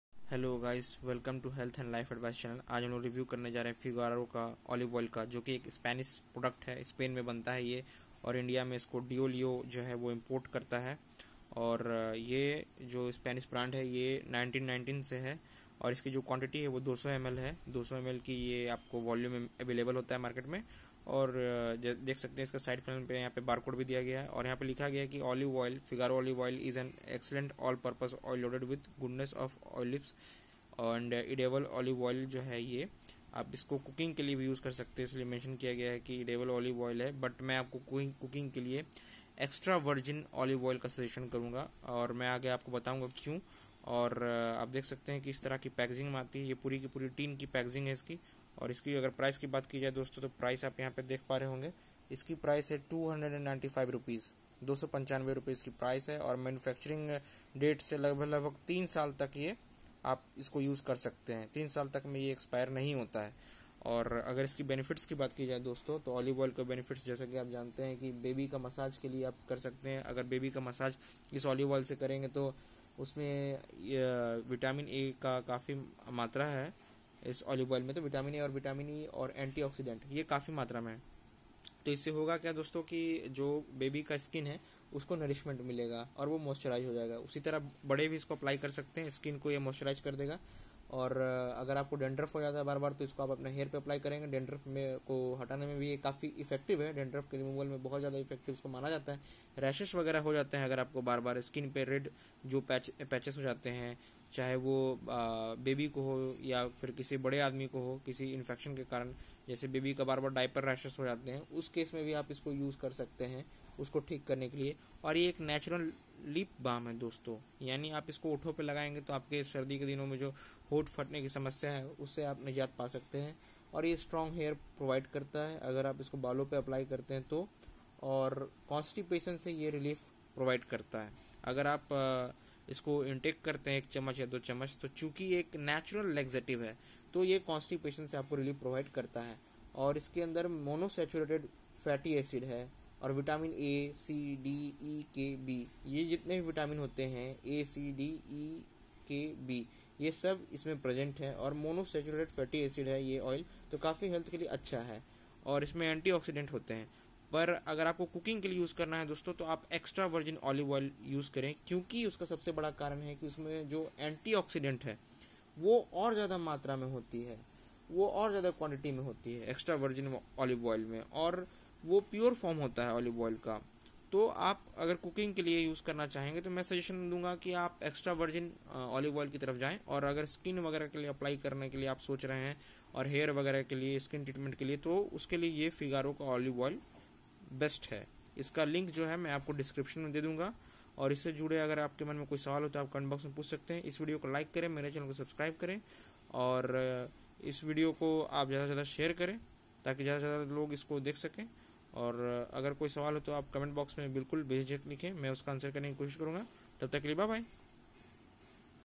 Figaro olive oil Spanish brand (200 ml) full review and complete details..
Figaro olive oil full use,full details and complete information..all about Figaro olive oil...